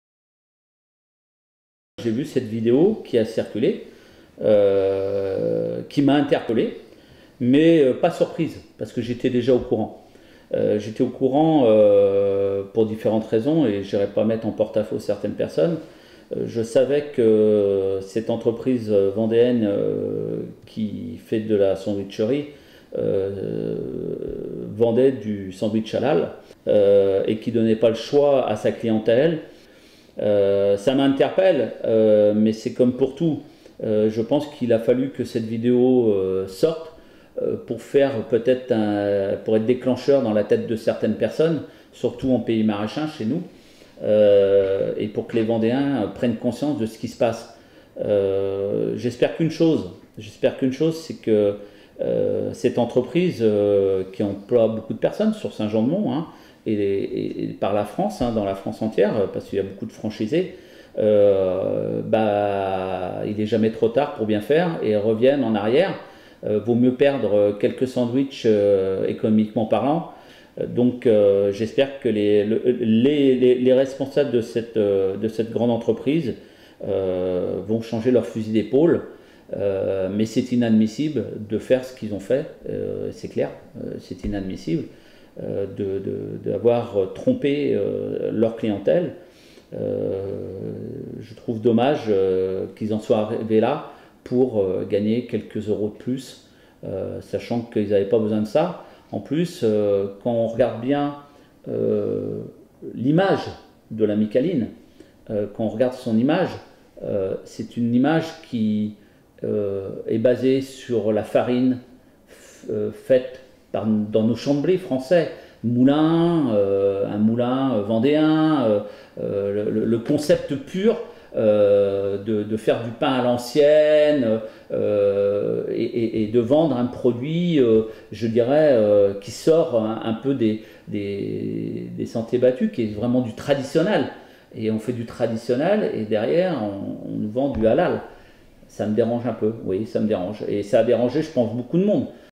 Eric Brondy , élu Municipal (85) /l'affaire Mie Câline halal.
Extrait du film de Eric Dick .Sortie prévue 2019.